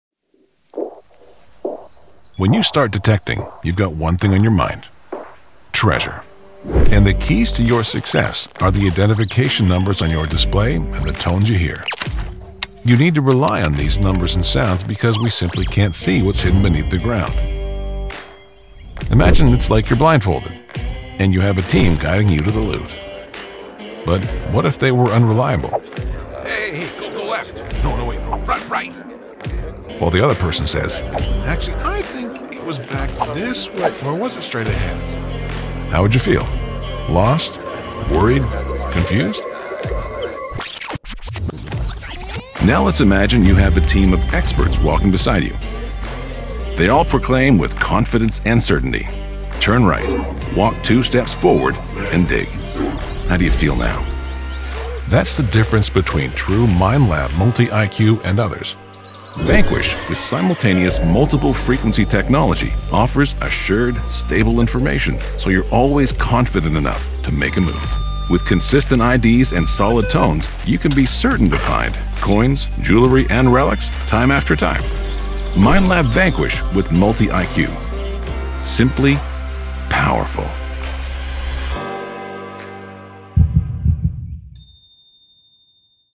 Minelab // Apex Animation
Our awesome client, Minelab, asked us to help them create an animation that demonstrated the USP of their new detector. We used a 2.5D animation style and some lush sound design to bring the messaging home.